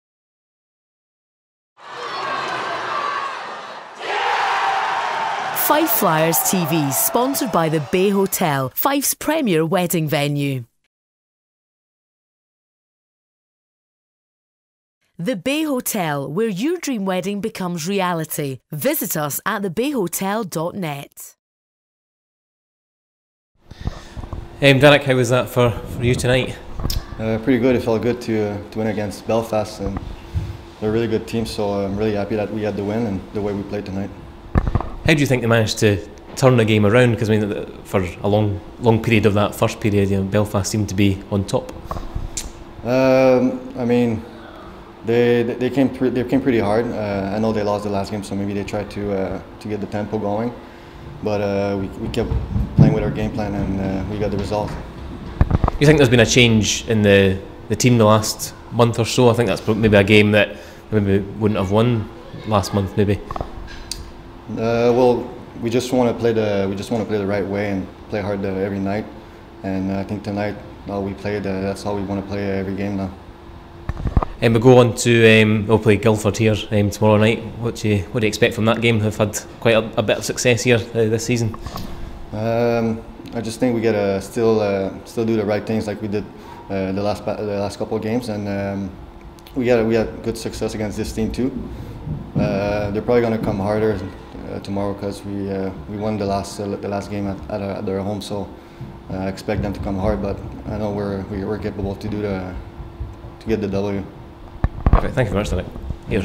POST MATCH | Danick Gauthier
The Flyers Forward talks to Flyers TV